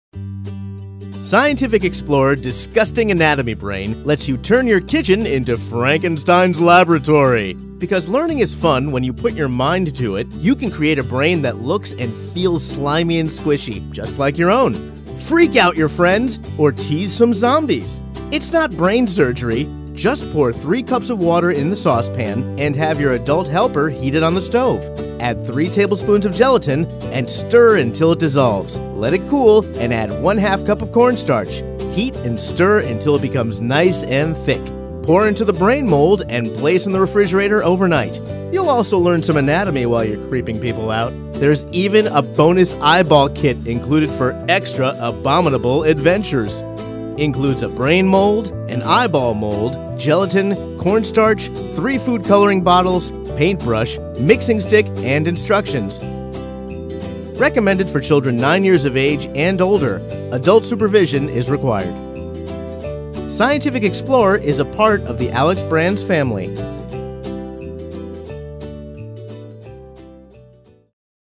Scientific Explorer Disgusting Anatomy Brain 0SA232TL
Scientific Explorer Disgusting Anatomy Brain lets you turn your kitchen into a Frankenstein laboratory! Create a brain that looks and feels slimy and squishy, just like your own. Freak out your friends, tease some zombies or act out your own horror movie. Also, learn some anatomy while you’re creeping people out. Bonus eyeball kit included for extra abominable adventures! Recommended for children 9 years of age and older.